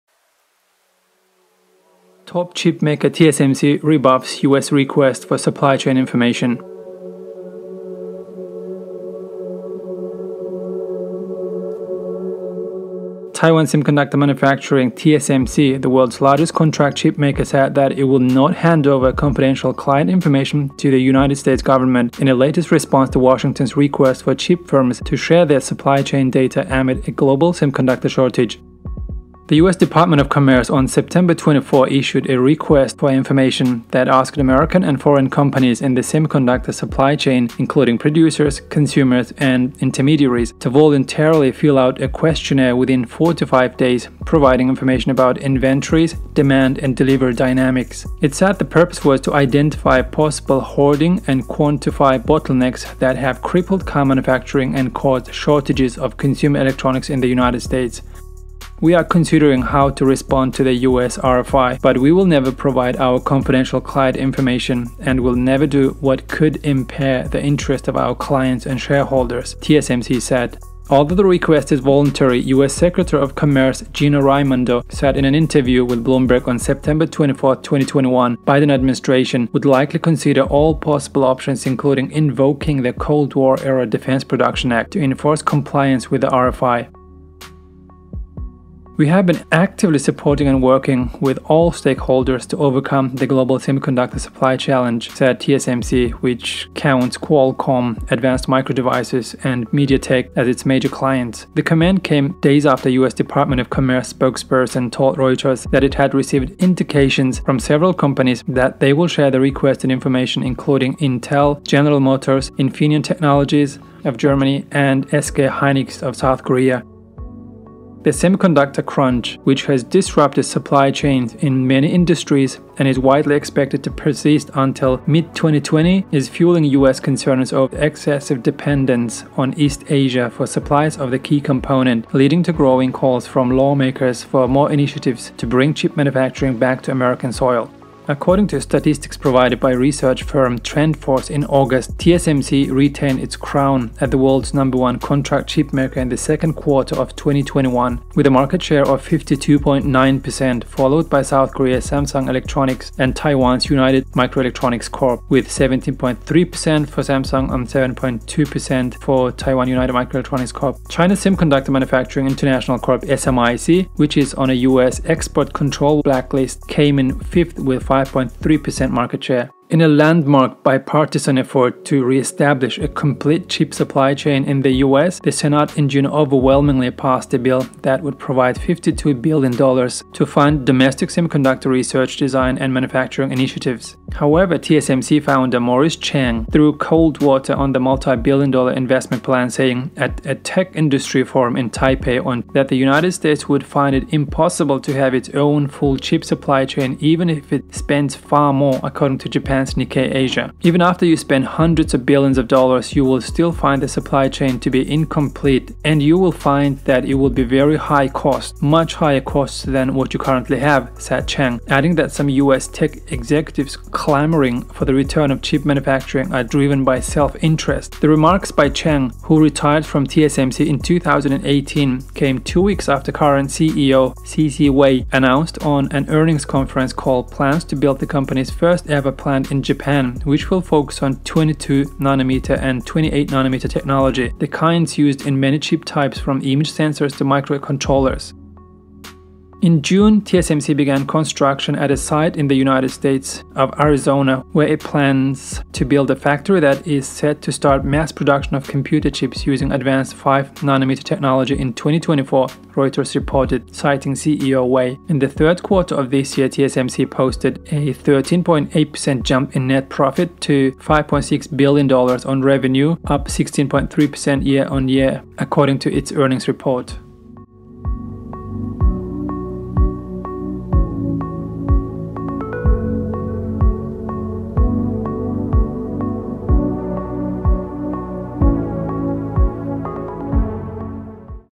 US vs China Chip Wars | TSMC fighting for its INDEPENDENCE
Reference :
Video Footages from TSMC and CNBC